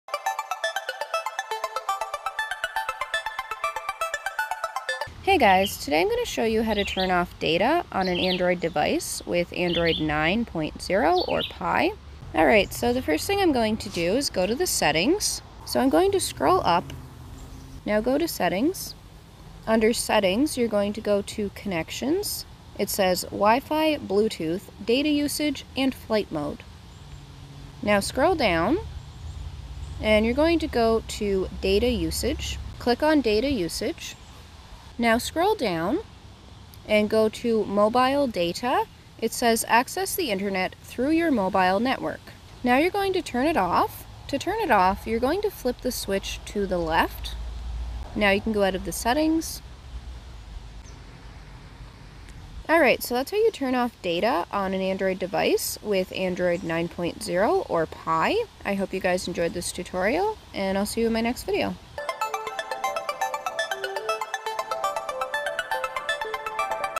How To Turn Off Data on android 9.0!! (Pie)
This video will show you how to turn off data on an android device with android 9.0 or pie

Thanks